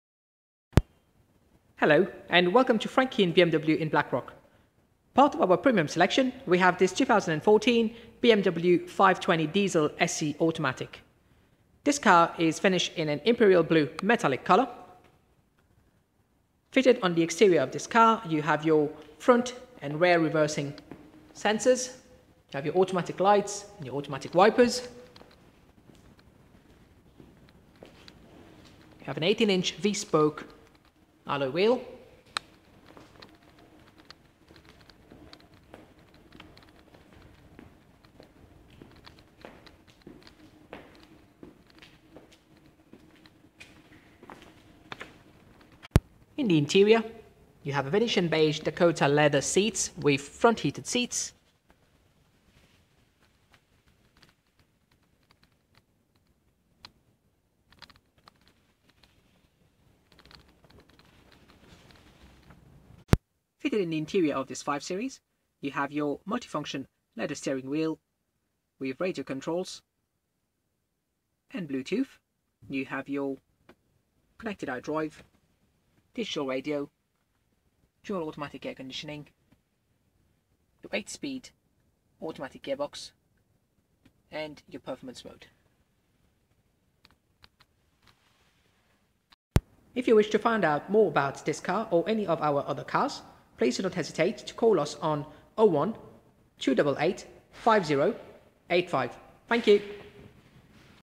141D31601 - 141D31601 BMW 520d SE Saloon
Here is a video of our 141D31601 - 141D31601 BMW 520d SE Saloon. Please contact Jason Cafolla to arrange a test drive today. BMW Blackrock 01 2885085